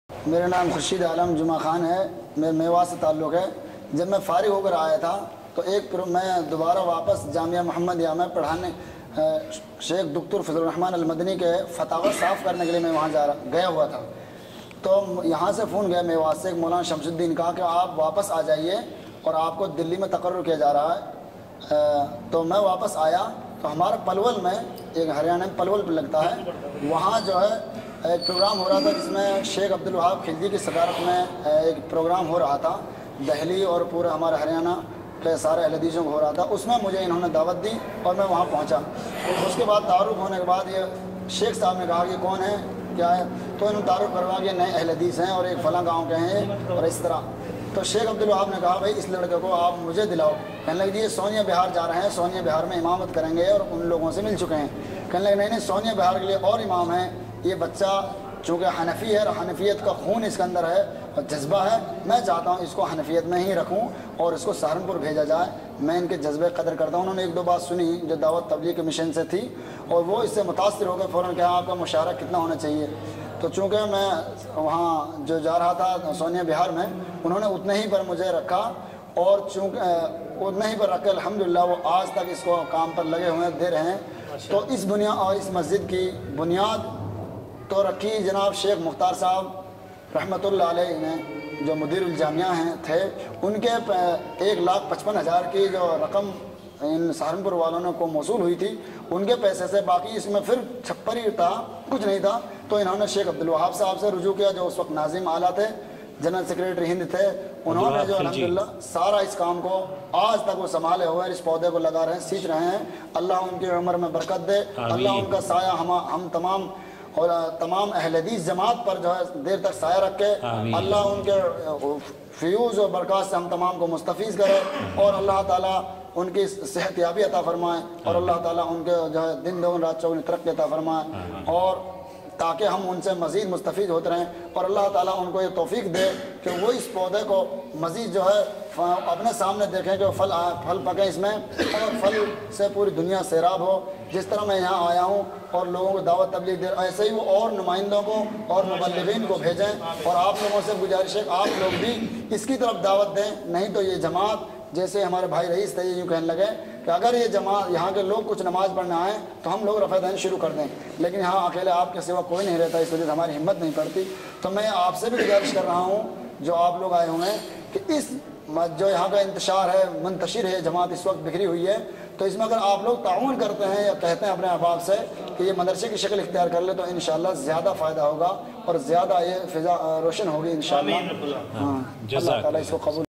Interview with Shaikh Khursheed Alam
Urdu Interview with a former Tableeghi, now a Salafi aalim of The Qur'an & Sunnah.